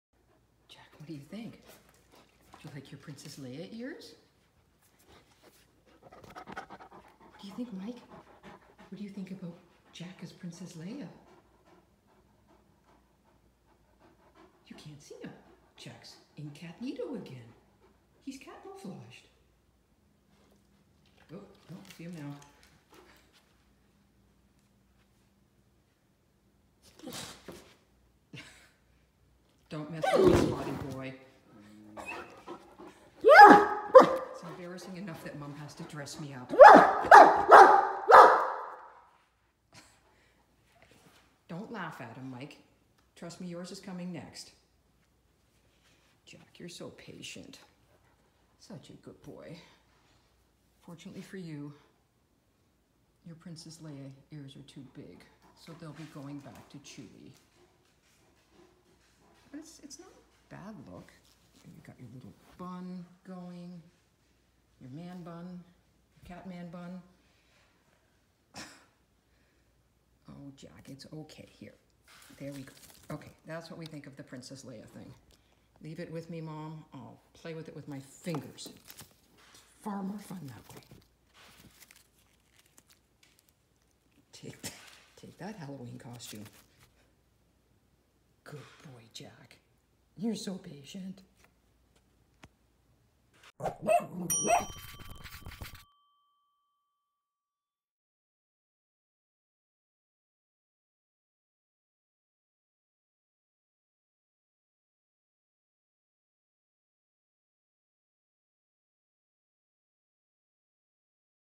Cranky Cat In Halloween Princess Leia Costume Swats Great Dane
Jack the Cat is a good natured guy about dressing up in costumes. most of the time. Watch and laugh as Jack is less than impressed with his Star Wars Princess Leia costume and gives Mikey a swat when he gets to up close and personal checking out his new hairdo!